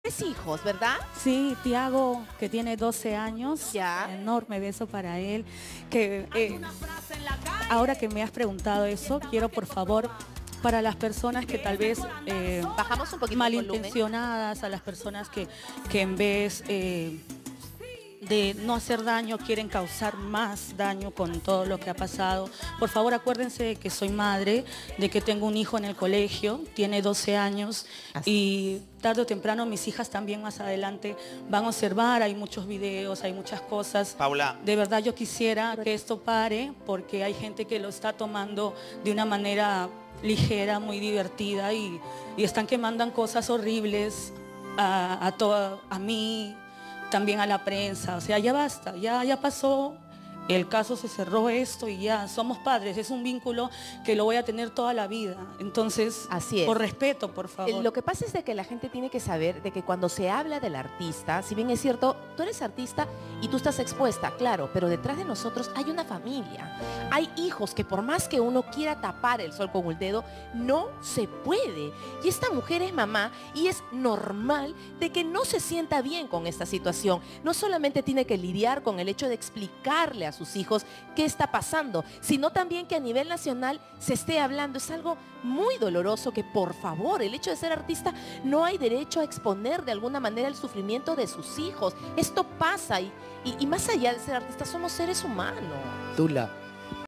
Paula Arias se conmovió en vivo al hacer este pedido tras separación
Paula Arias reapareció ante cámaras y esto dijo tras terminar con Tomás Medrano.